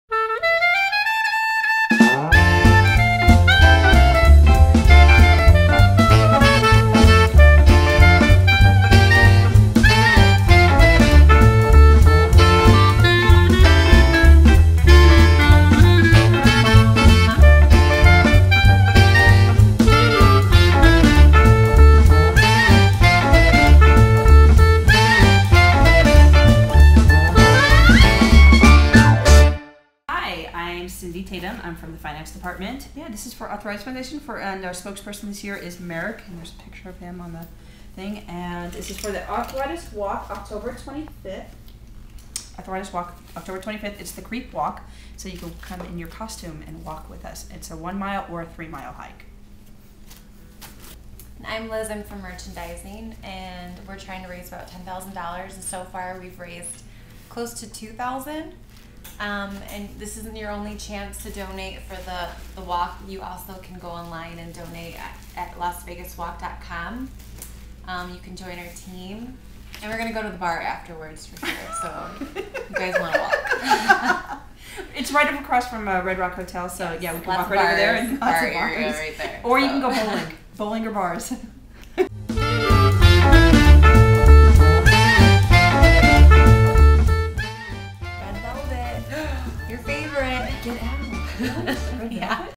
Bake Sale
Today we sold baked goods to help donate as much money as possible to the arthritis foundation.